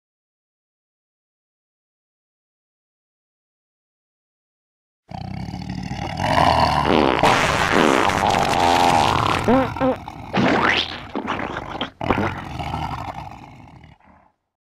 The Birth of Craig
this video documents a rhino birthing a human-rodent hybrid through it's intestinal tract. this first ever event thrilled scientist and dawned a new species called Craig. Craig nestled in fecal matter until his instinct to nurse took over. unfortunately, the rhino was a male unable to produce milk from it's nipples. this intelligent new life form adapted quickly and suckled it's nourishment from the rhinos penis finding all the proteins needed to survive.